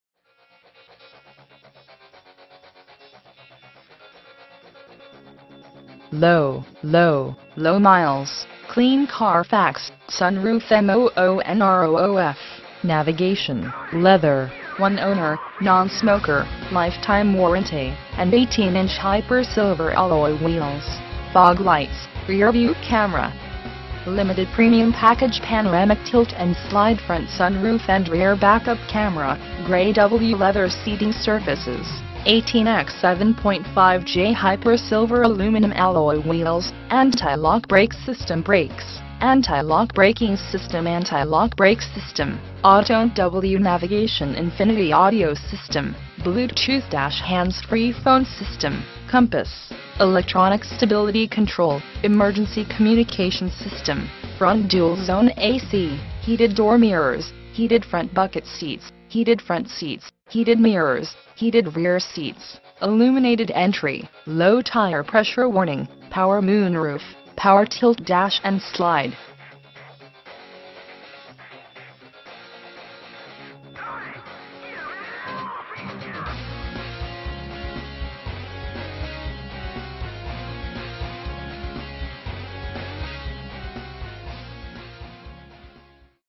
2013 Hyundai Sonata - Eastern Shore Hyundai - Daphne, AL 36526 - P5182
Eastern Shore Hyundai
29736 Frederick Boulevard

For Sale: 2013 Hyundai Sonata
VIN: 5NPEC4AB9DH666206
Engine: 2.0L 4-Cylinder DGI Turbocharged
Drivetrain: FWD
Transmission: 6-Speed Automatic with Shiftronic
Mileage: 17,173
Color: Harbor Gray Metallic (exterior) Gray (interior)